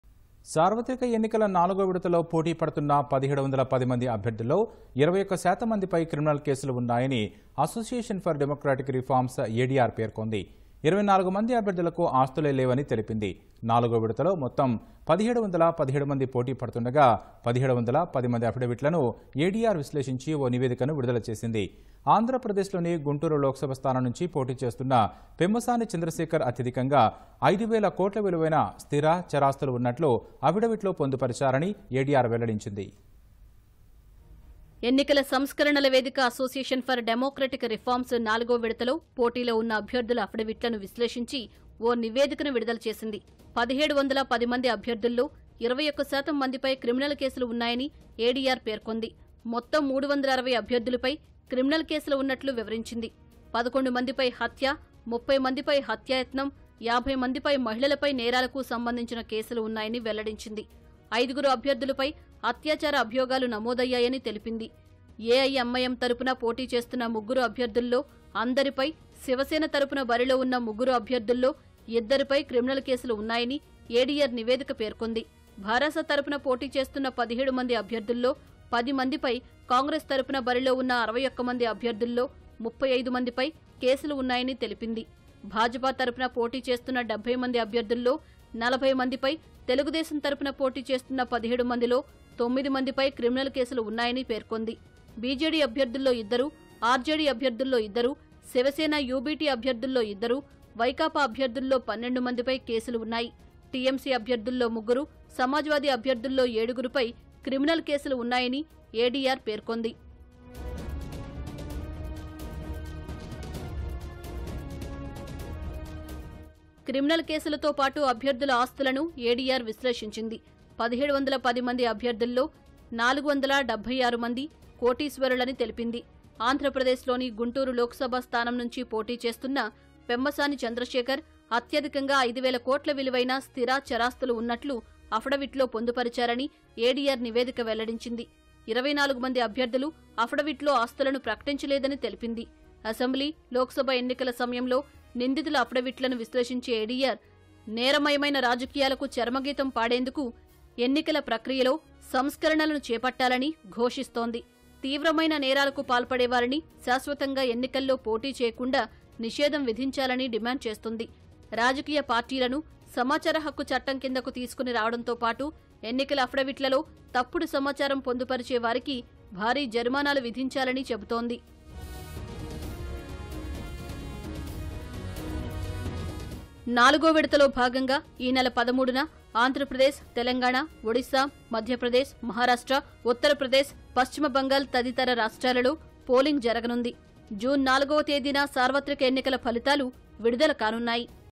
360 మందిపై క్రిమినల్‌ కేసులు | Criminal Cases on 360 Contestants | 4th Phase of LS Polls @ADR Report
సార్వత్రిక ఎన్నికల నాలుగో విడతలో పోటీ పడుతున్న1710 మంది అభ్యర్థుల్లో. 21 శాతం మందిపై క్రిమినల్‌ కేసులు ఉన్నాయని అసోసియేషన్‌ ఫర్‌ డెమోక్రటిక్‌ రిఫార్మ్స్‌-ADR పేర్కొంది. 24 మంది అభ్యర్థులకు ఆస్తులే లేవని తెలిపింది. నాలుగో విడతలో మెుత్తం 1717 మంది పోటీ పడుతుండగా..1710 మంది అఫిడవిట్లను ADR విశ్లేషించి ఓ నివేదికను విడుదల చేసింది. ఆంధ్రప్రదేశ్‌లోని గుంటూరు లోక్‌సభ స్థానం నుంచి పోటీ చేస్తున్న పెమ్మసాని చంద్రశేఖర్‌ అత్యధికంగా 5 వేల కోట్ల విలువైన స్థిర, చరాస్తులు ఉన్నట్లు అఫిడవిట్‌లో పొందుపరిచారని ఏడీఆర్‌ వెల్లడించింది.